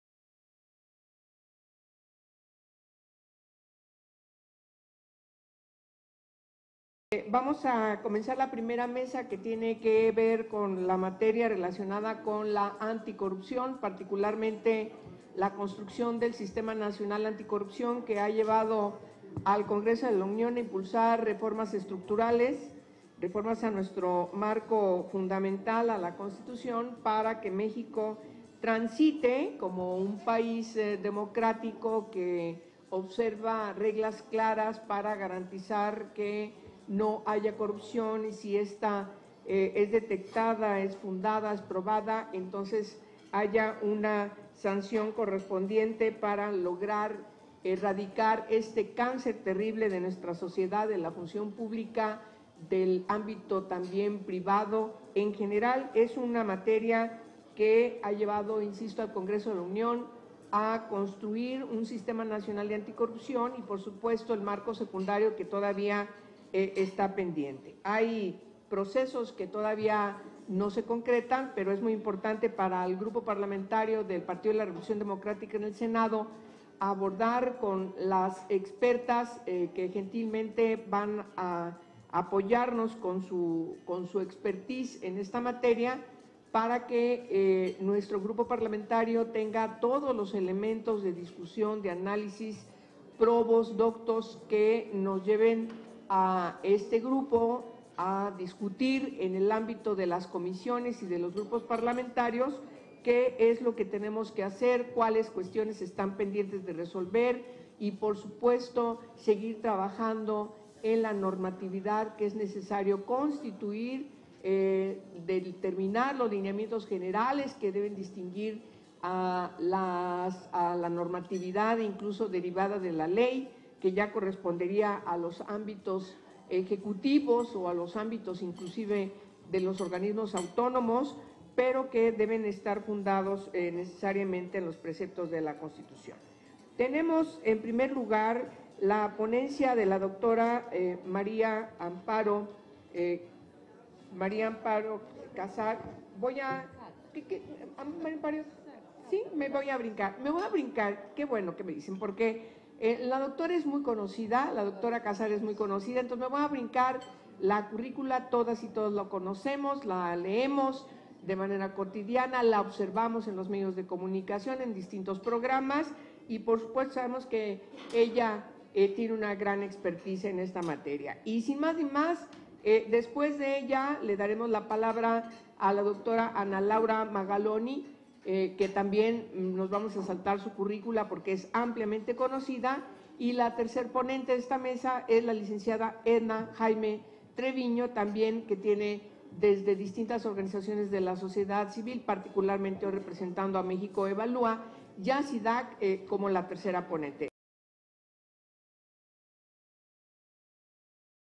Intervención de Angélica de la Peña Gómez en la Mesa 1. SNA
La senadora Angélica de la Peña Gómez, participó en la primera mesa de la PlenariaPRD señaló que la construcción del SNA ha llevado al Congreso a impulsar reformas estructurales para que México transite como un país democrático con reglas claras.